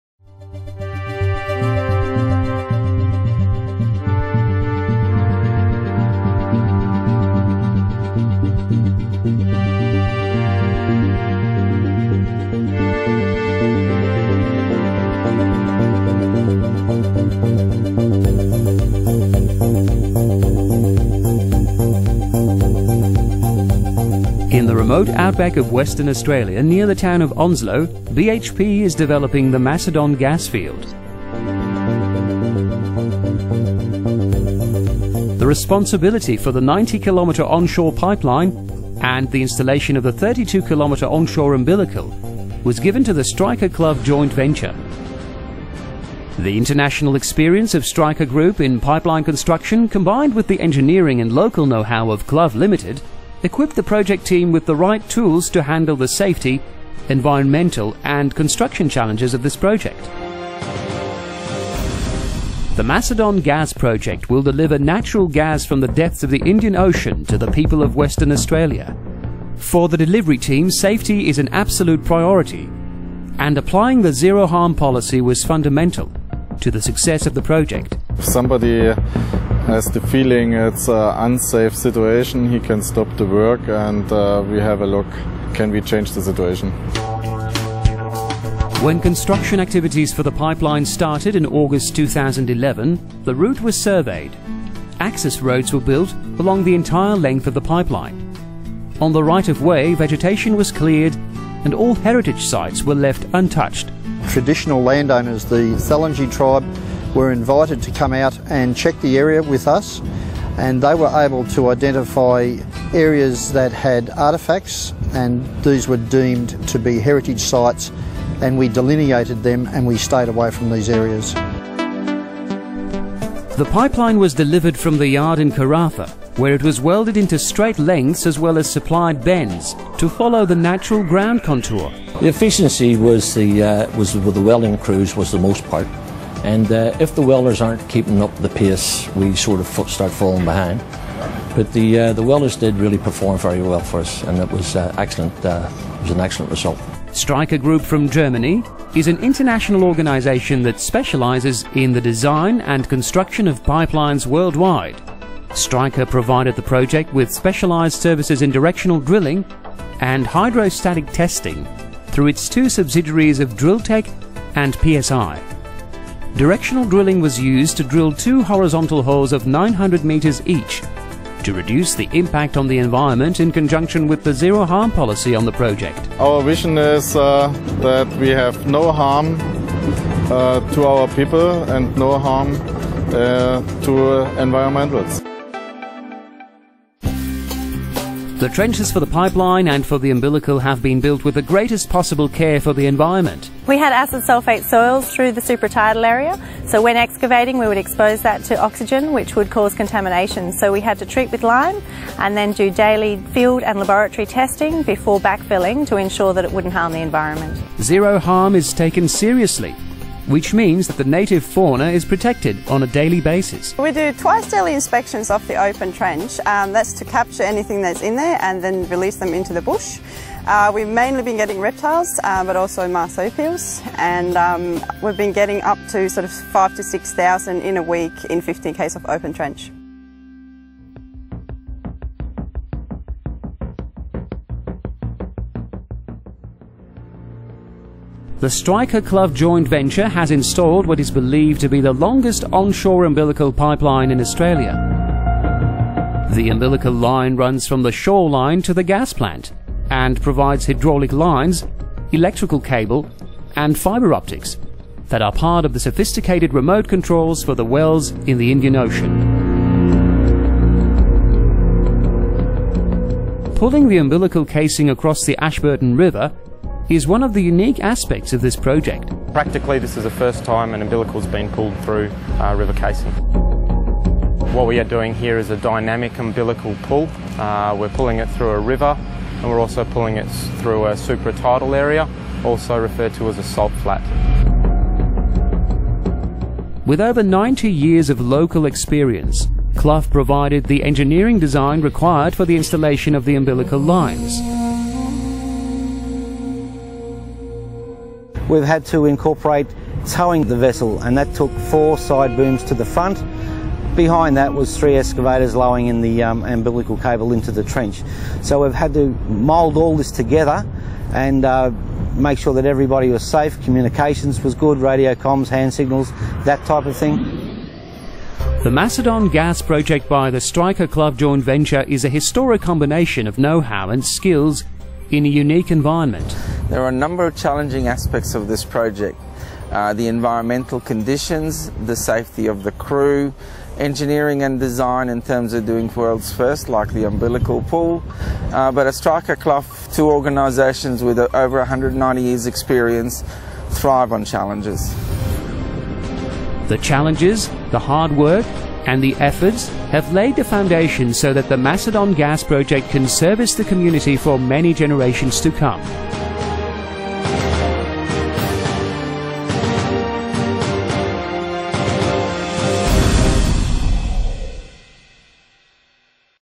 Macedon Pipeline Project - Australia
Together with the Australian engineering company Clough, STREICHER Australia was involved in a pipeline project in Western Australia, 1,800 km north of Perth. On behalf of the energy and resources company BHP Billiton, 82 km of gas pipeline DN 500 were laid between June 2011 and May 2012.